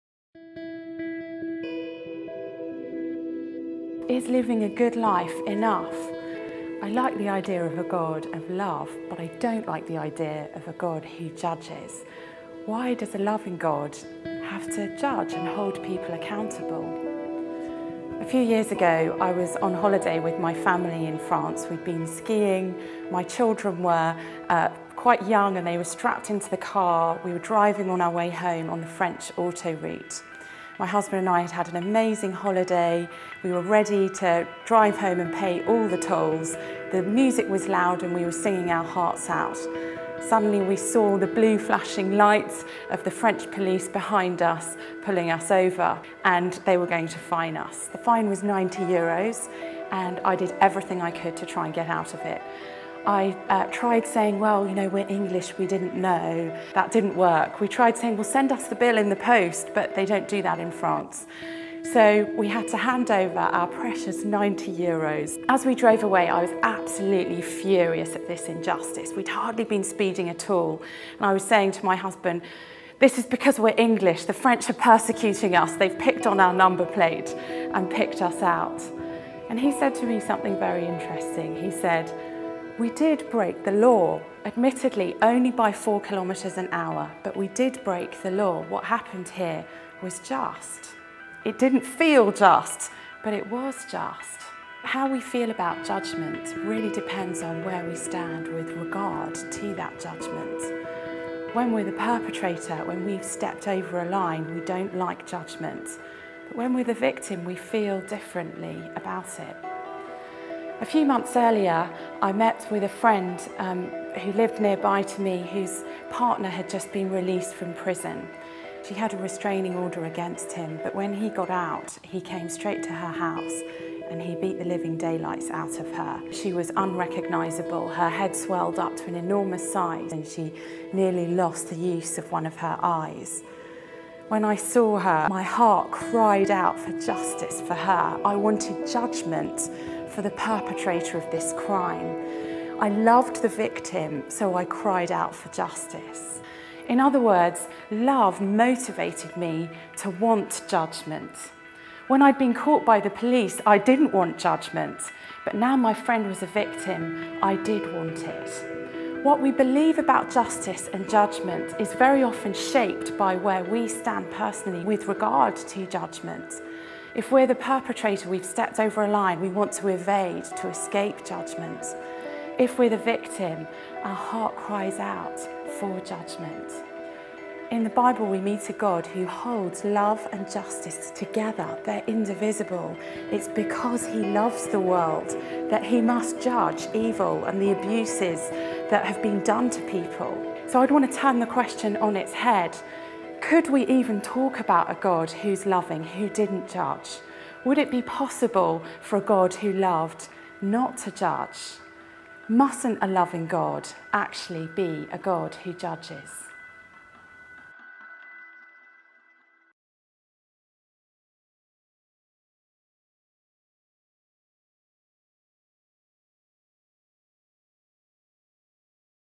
Uncover Luke - Is living a good life enough?
Isn’t living a good life enough?  Isn’t there a god of love?  Why does there have to be this idea of judgment and accountability?

Amy Orr Ewing argues that we feel very differently about judgment when we’re the perpetrator, than when we’re the ones who have stepped over a line. We don’t like judgment when we’re guilty but when we’re the victim, we feel differently about it.